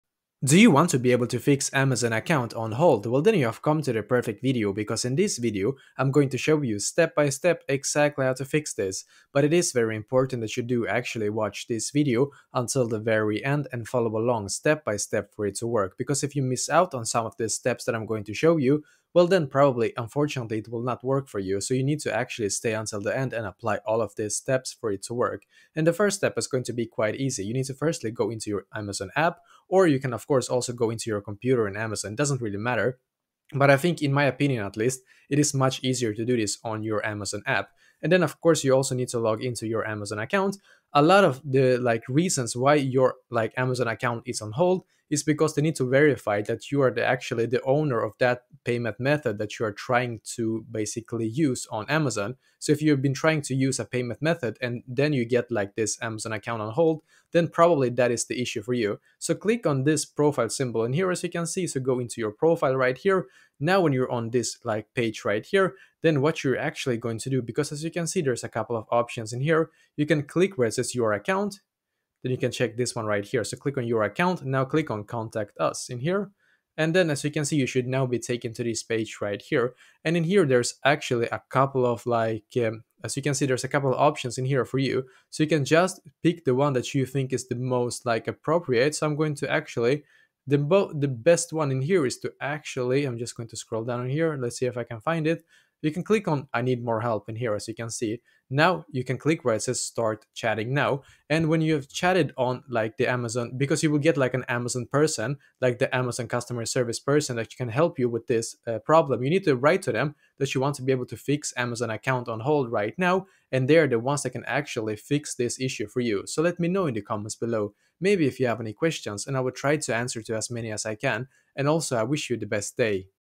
How To Fix Amazon Account on Hold (Full Guide)
If you want to fix amazon account on hold then this video will help you with this!